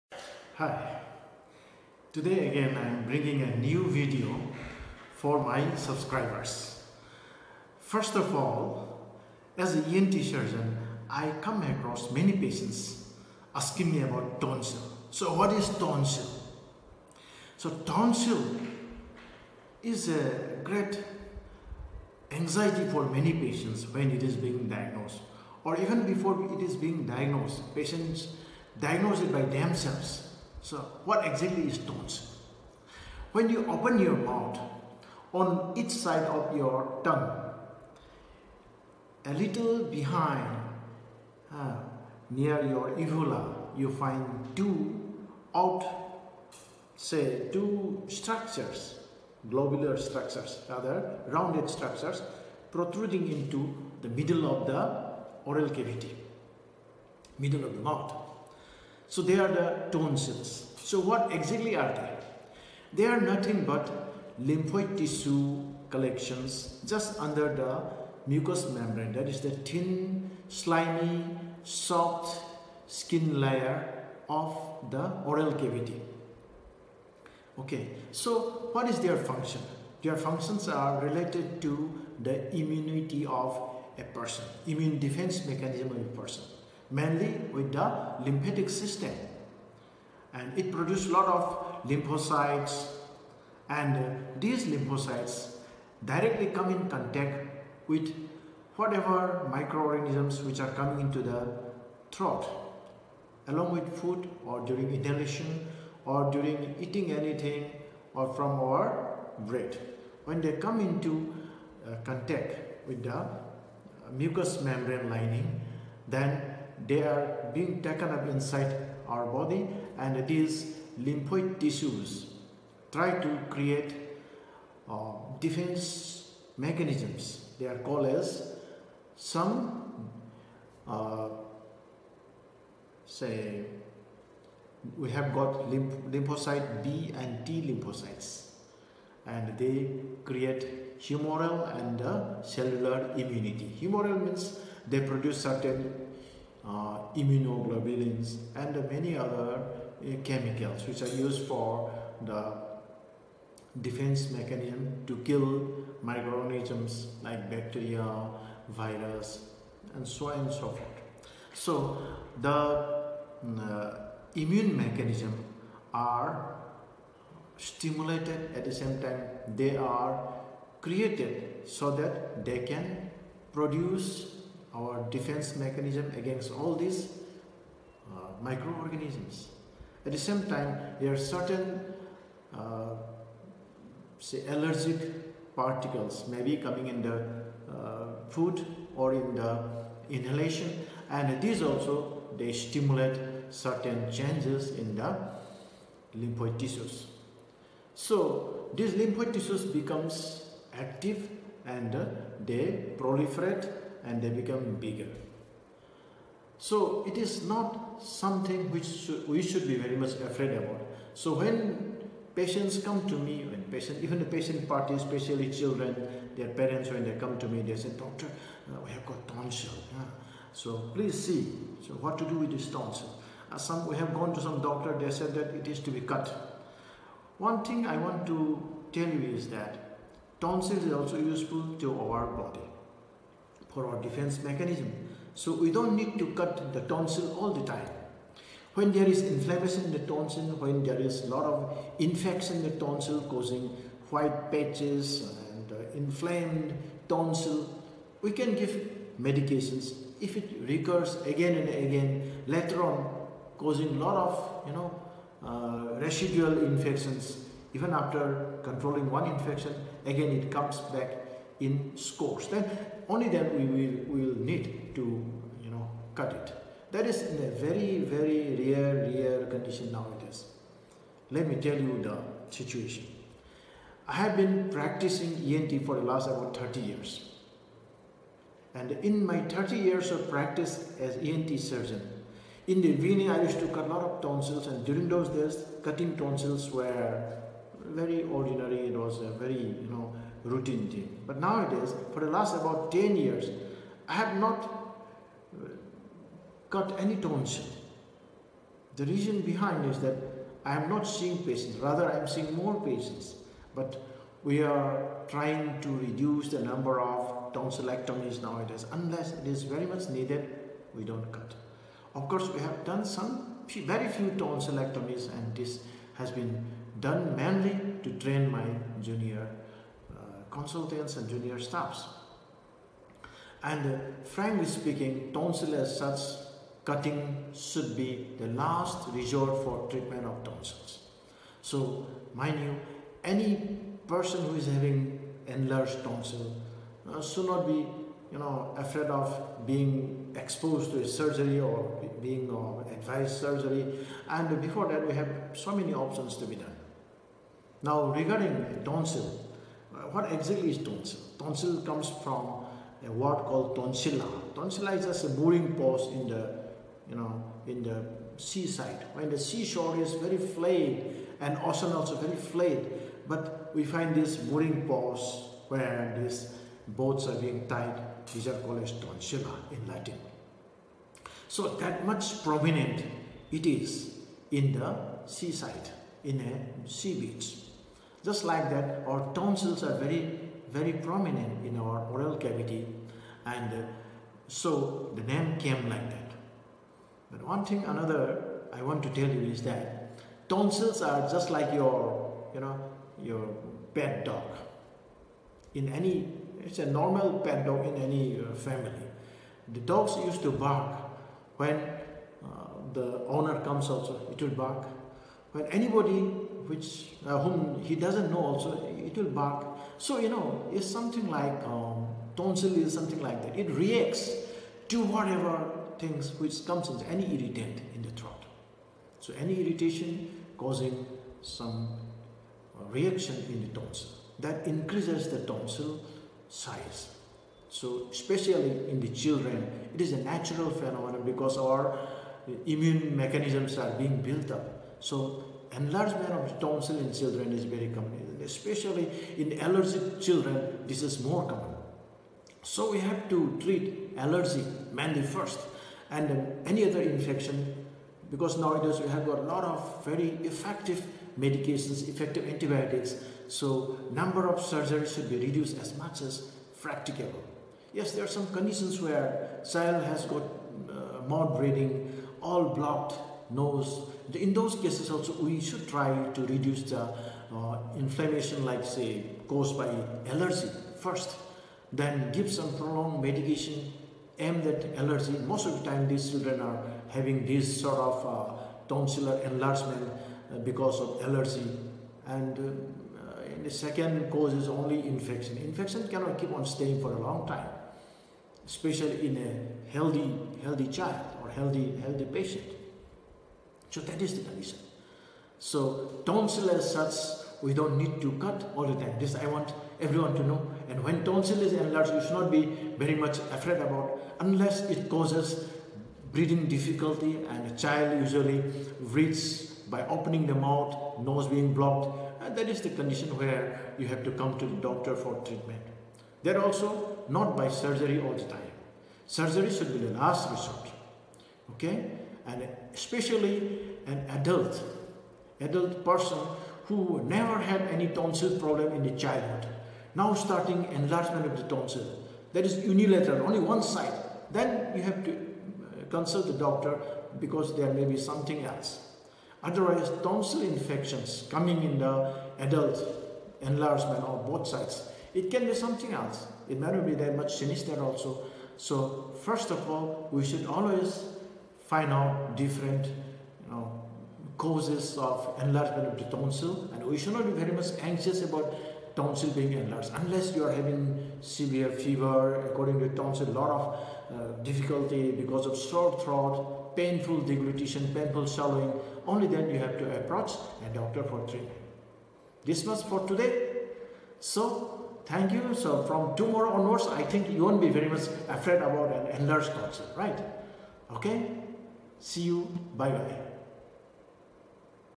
What is Tonsil?
Many people are afraid of enlarged tonsil. This is a little step in making everyone understand about tonsil, its function, enlargement & need for surgery. Tonsil is a very useful organ and we should consult a ENT surgeon to remove our fears of its enlargement.